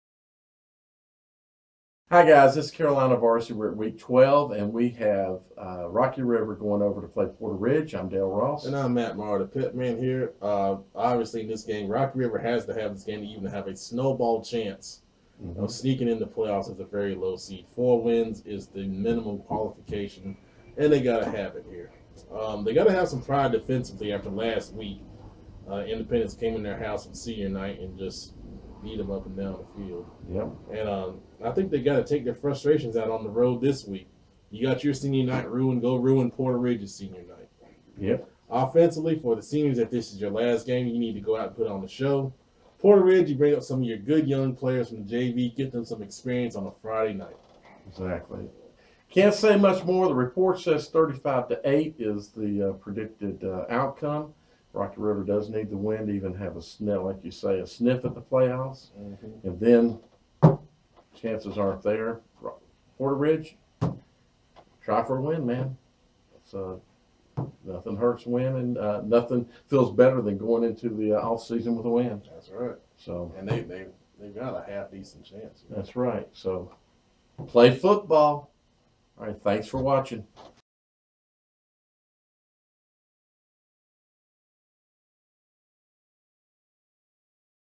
2015 Week 12 Rocky River Vs Porter Ridge
Matt and Dale discuss the Rocky River Vs Porter Ridge Game